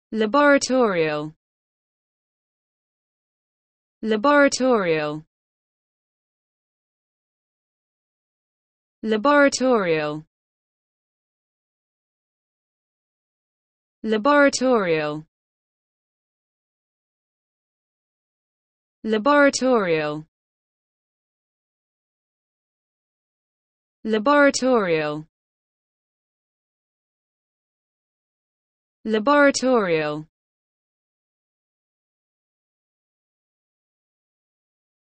the correct pronunciation of labyrinthici in English.
This video shows you how to pronunciation labyrinthici in English.